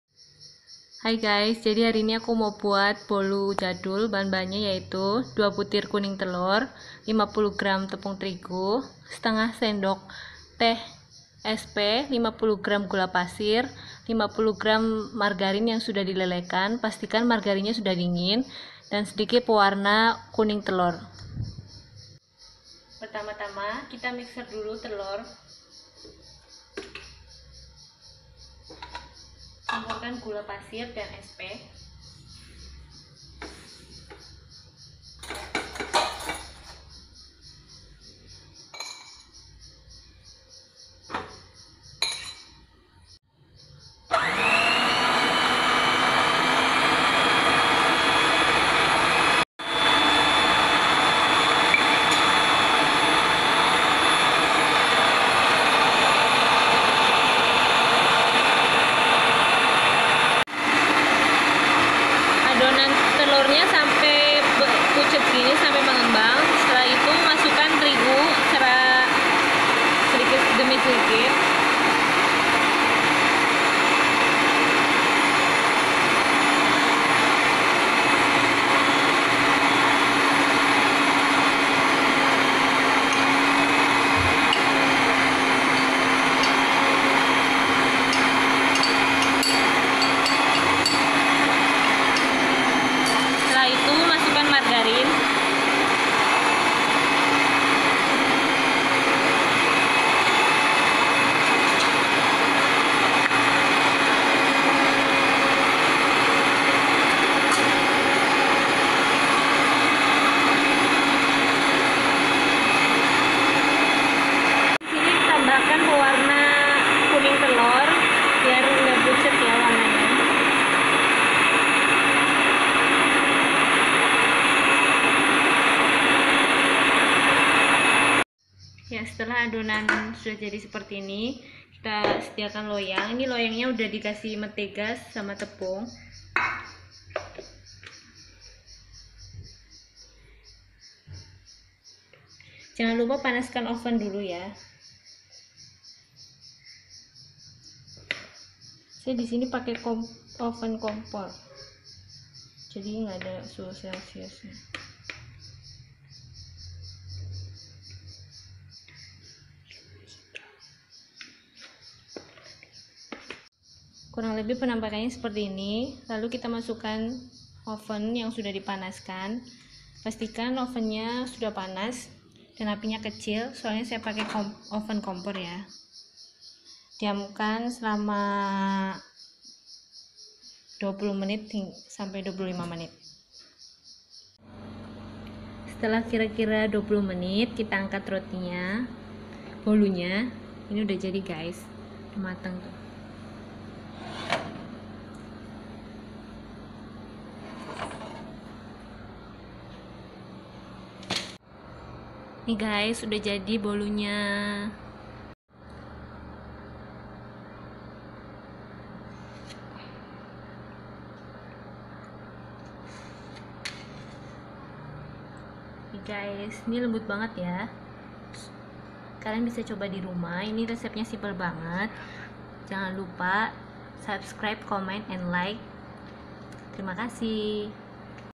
Resep simpel Bolu Jadul enak, lembut dan ekonomis
Resep bolu jadul
Bahan:
2 butir telur utuh
1 sdt sp
50 gram gula pasir
50 gram tepung trigu
50 gram margarin (di lelehkan)